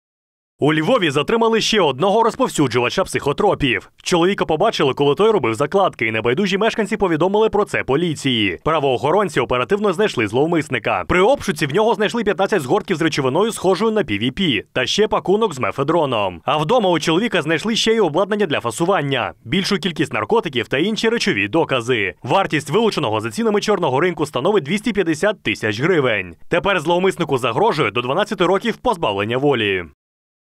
У Львові затримали ще одного розповсюджувача психотропів
Під час обшуку в нього знайшли 15 згортків з речовиною схожою на «PVP» та ще пакунок з «мефедроном». А вдома у чоловіка знайшли ще й обладнання для фасування, більшу кількість наркотиків та інші речові докази.